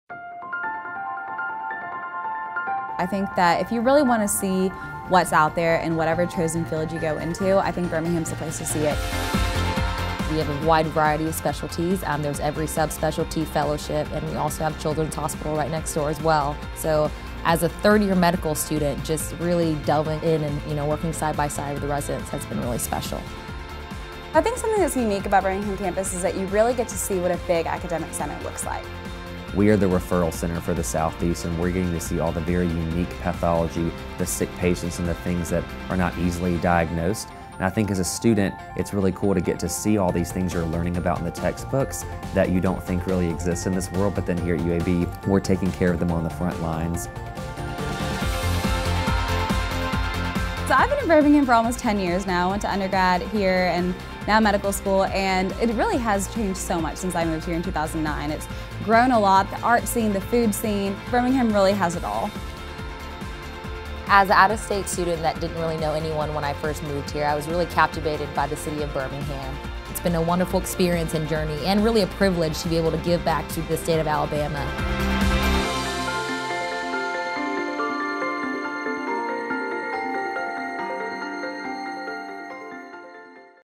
UAB Heersink School of Medicine
The Heersink School of Medicine is a four-campus medical school, with UAB serving as the main campus and three regional campuses located in Huntsville, Montgomery and Tuscaloosa. All students spend their first two years of medical school in Birmingham and are located at one of the four campuses for their clinical years. Find out a bit more about our Birmingham campus in this video.